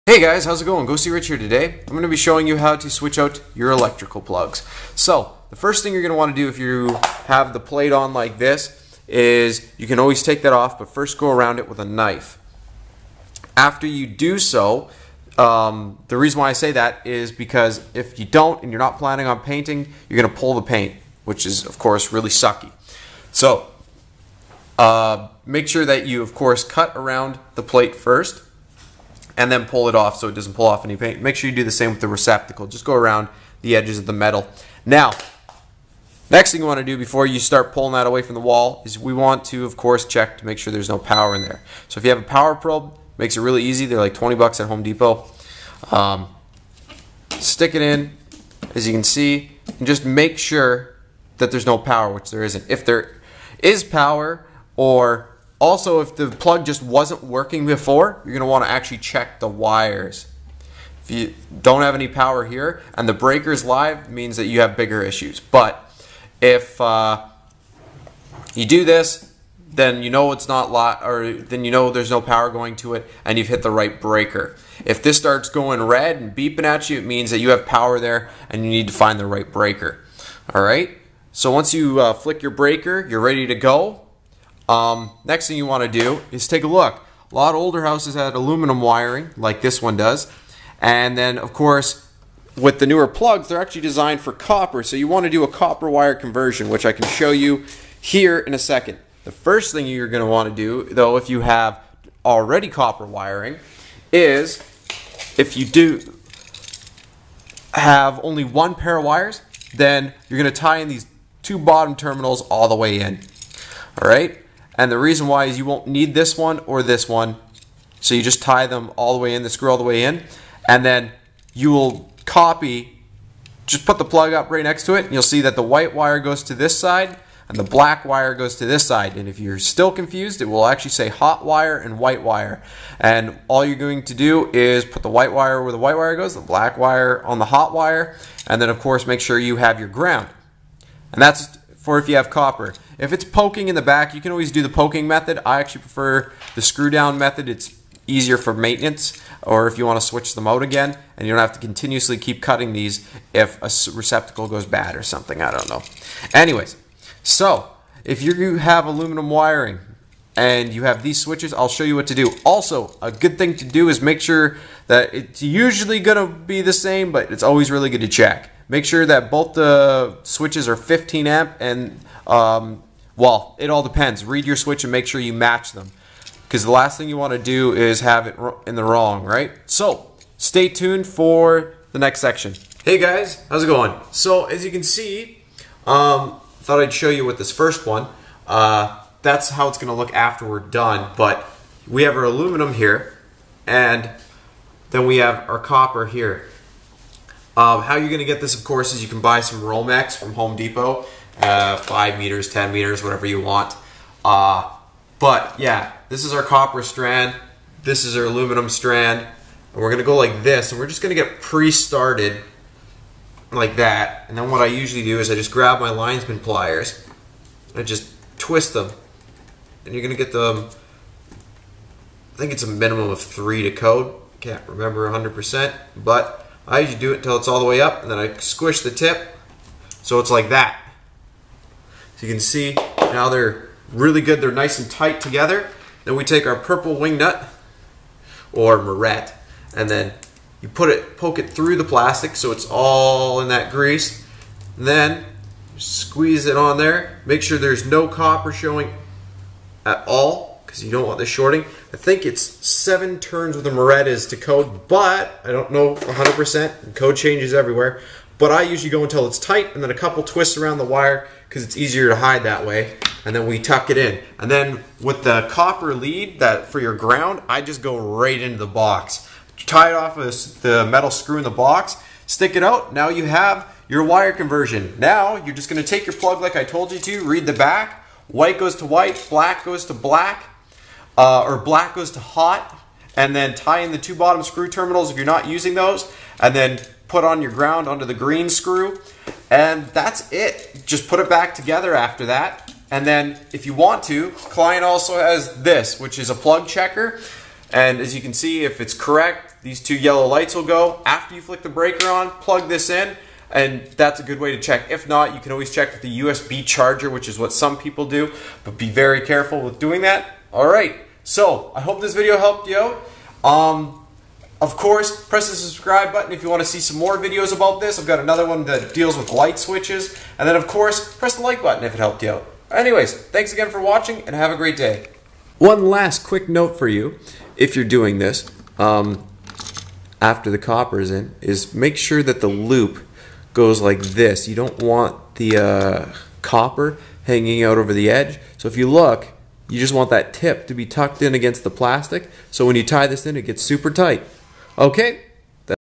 HOW TO EASILY REPLACE ELECTRICAL PLUG SOCKET OUTLET COPPER OR ALUMINUM WIRING
Hey guys i replaced all the switches in my house i hope this tutorial shows you how easy it is to do so and how to adapt old aluminum wiring to work with the copper wiring outlets sold in stores